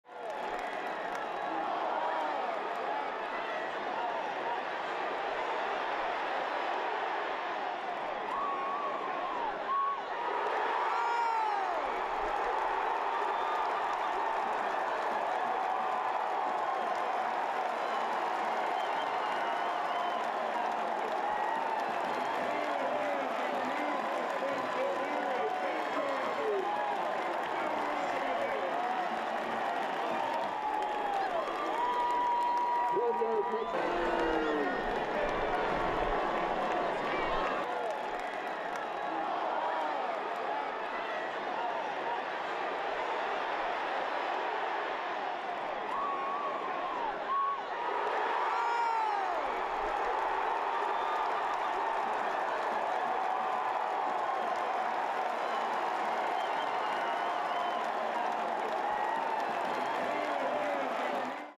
Taylor Heinicke Ridiculous TD vs. JMU
Old Dominion quarterback Taylor Heinicke threw a crazy touchdown pass to Larry Pinkard on 4th down against James Madison back in 2011. This was ODU's first game against JMU, Monarchs won the game.